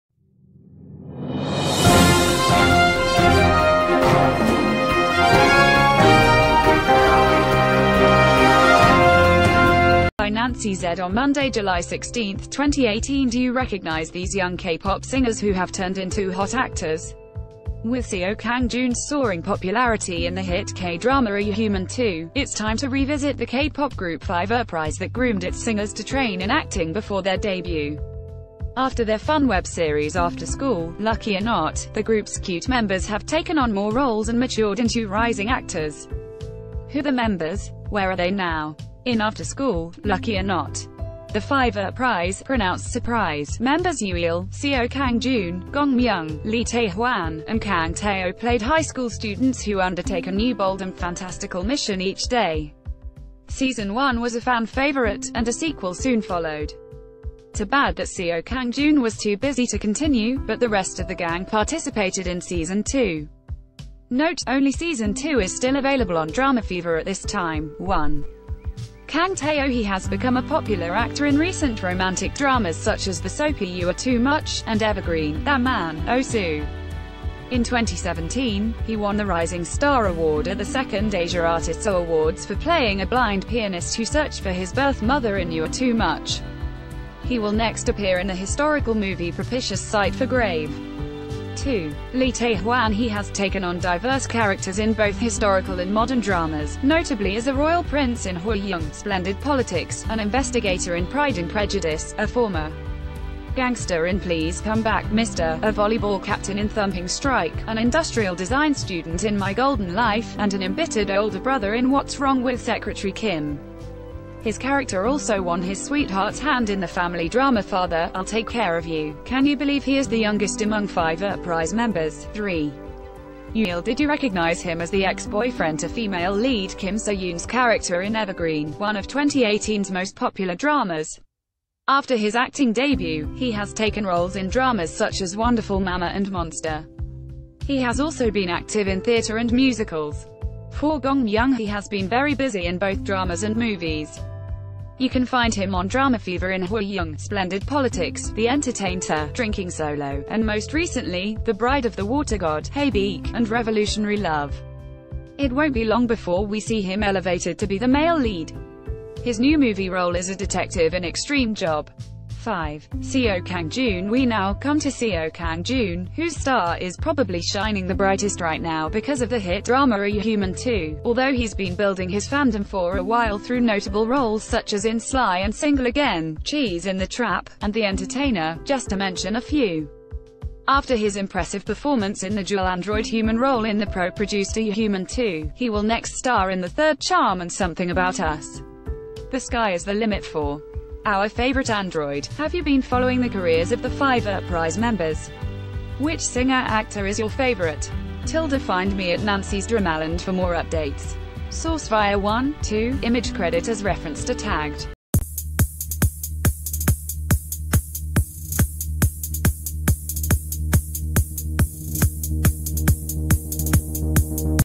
Seo Kang Joon and the adorable K-pop actor-idol group 5urprise: Where are they now?
by Nancy Z on Mon, Jul 16, 2018 Do you recognize these young K-pop singers who have turned into hot ...